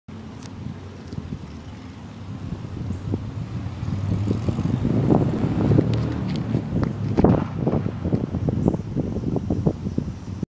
Discus training 2
Summer Training, 95 foot slown down spin working on technique